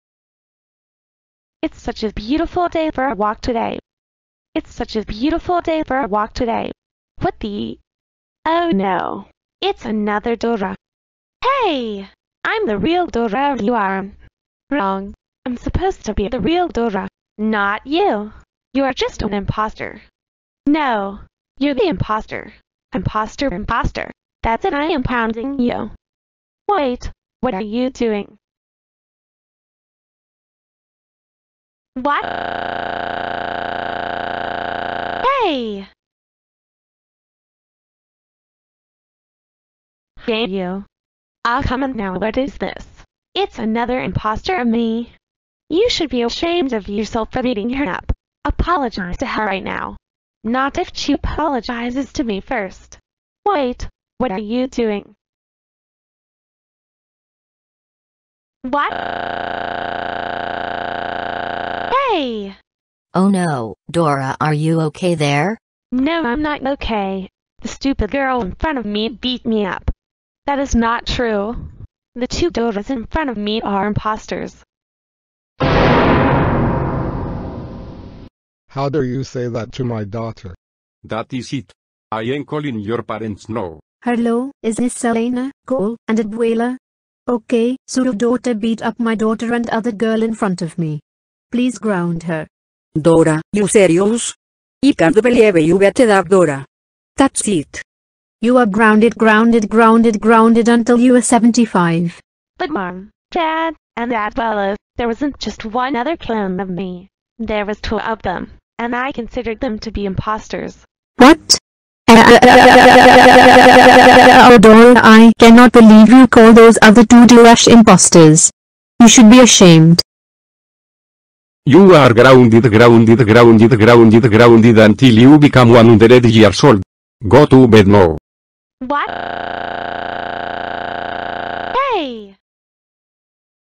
Dora Beats Up 2 Doras And Calls Them Imposters
Dora beat up her pre-AKS2003 clone, as well as her GA4S clone.
Credit to everyone and Brendan Barney!